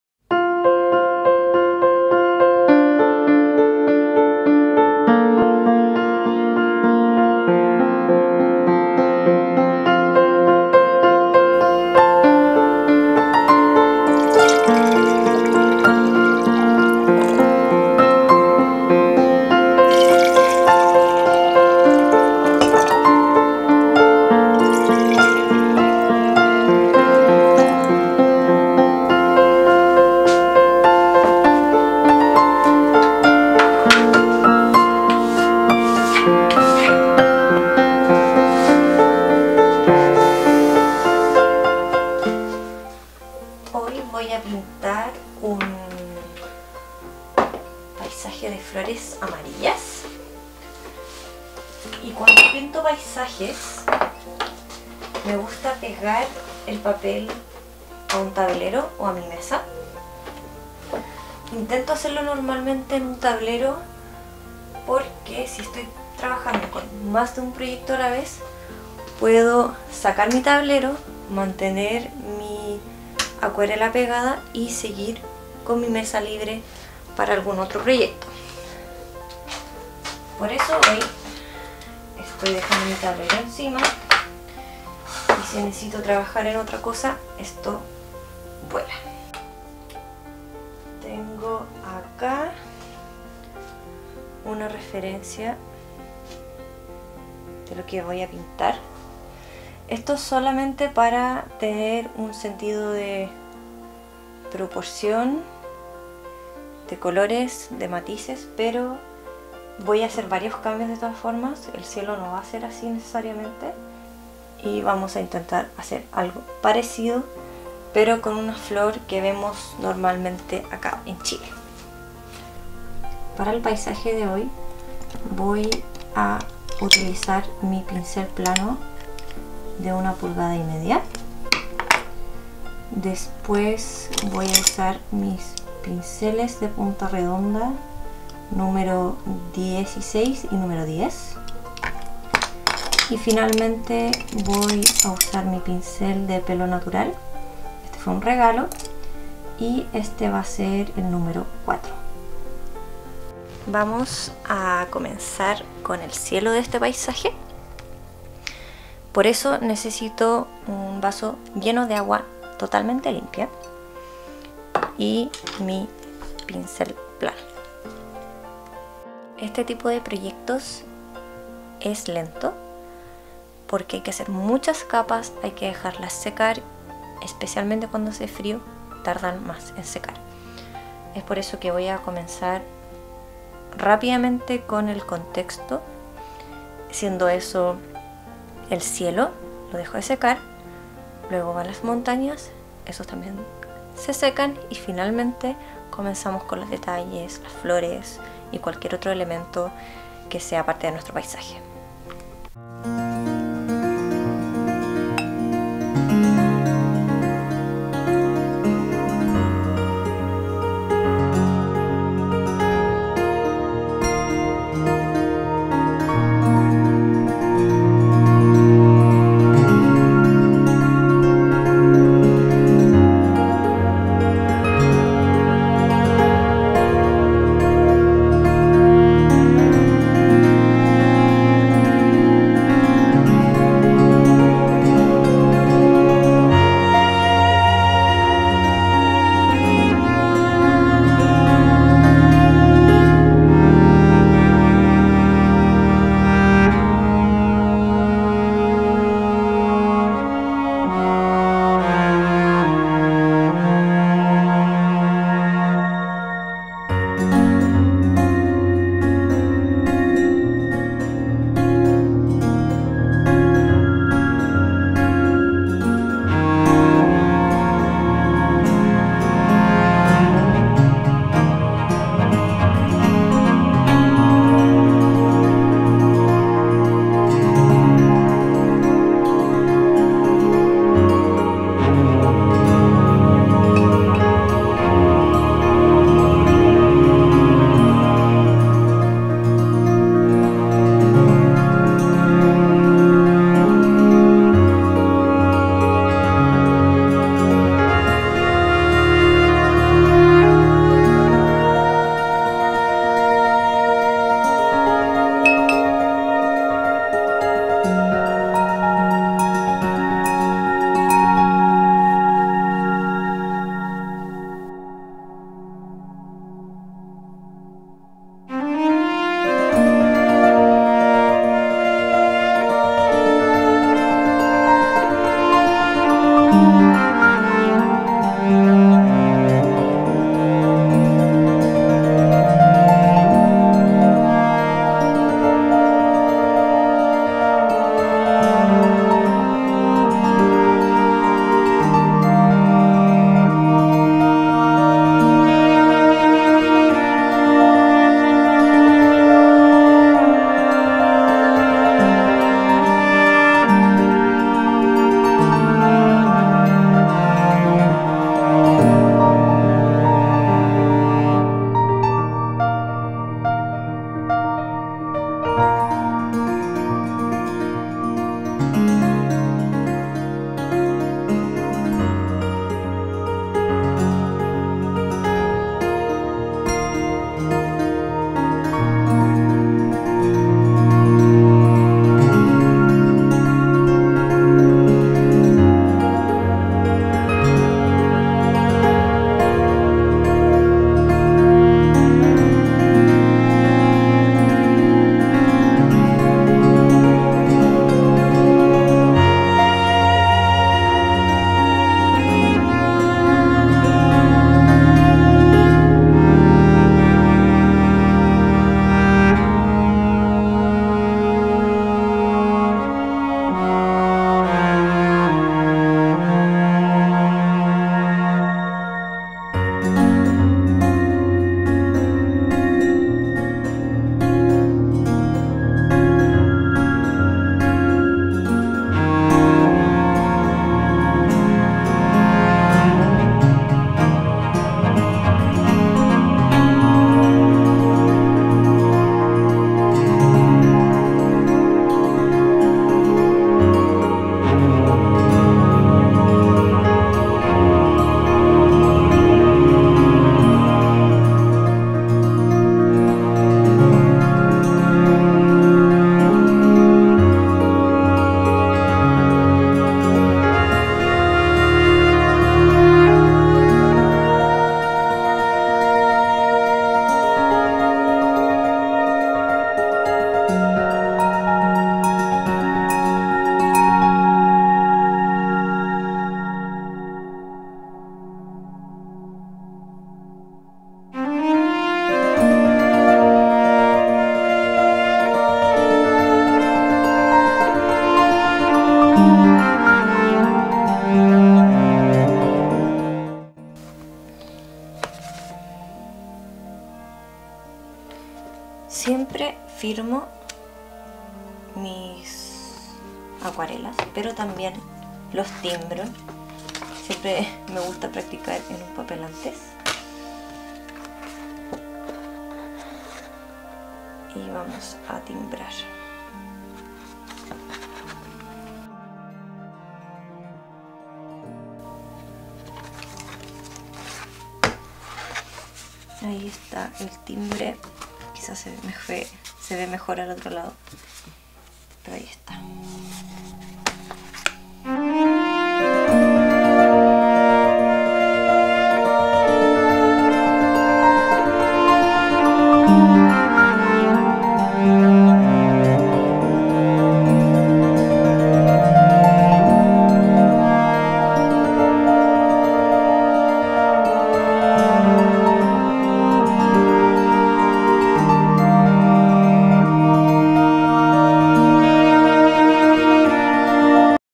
Un día en el taller - California Poppies
Un día en el taller pintando un campo de California Poppies.